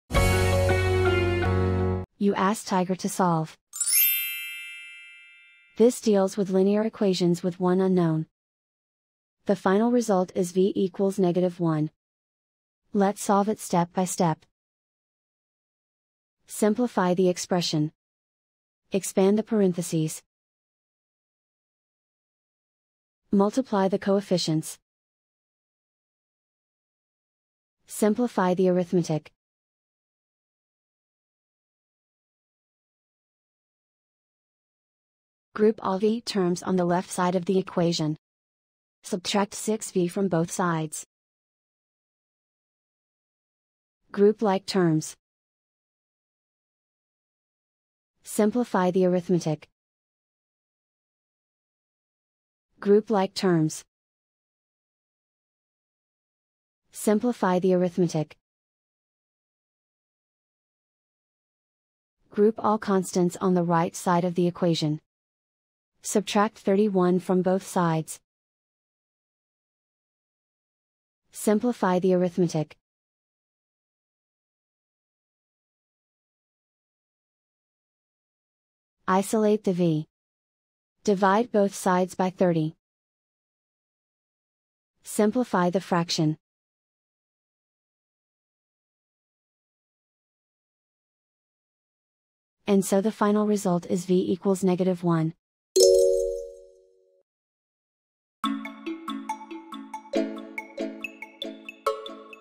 Linear equation with one unknown: Solve 6(6v+6)-5=1+6v step-by-step solution
Solving 6(6v+6)-5=1+6v using the linear equation with one unknown solver

for a step-by-step and more tips and tricks for dealing with linear equations with one unknown.